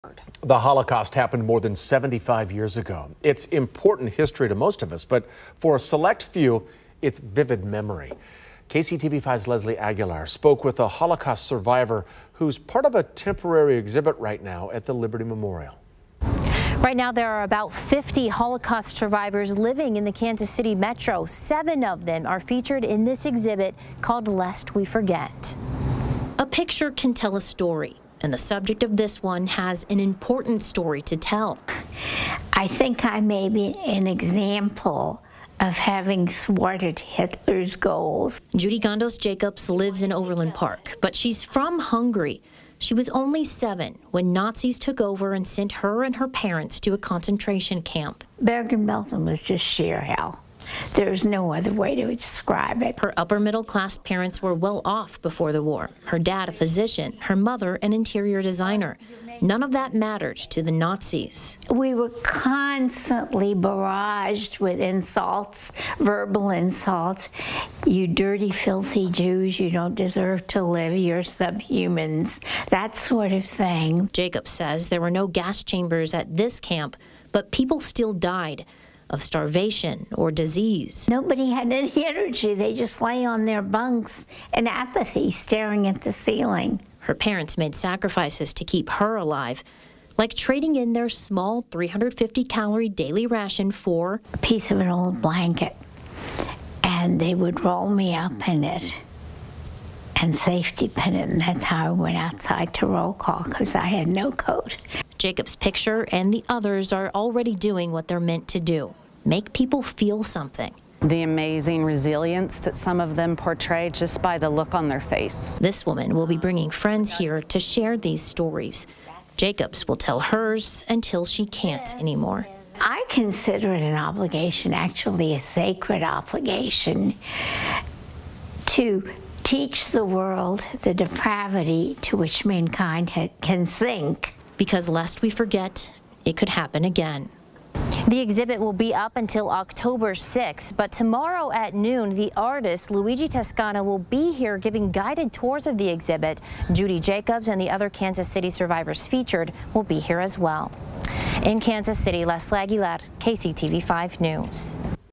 Local Holocaust survivor part of exhibit at Liberty Memorial
The Holocaust happened more than 75 years ago and it’s important history to most of us. However, for a select few, it’s a vivid memory.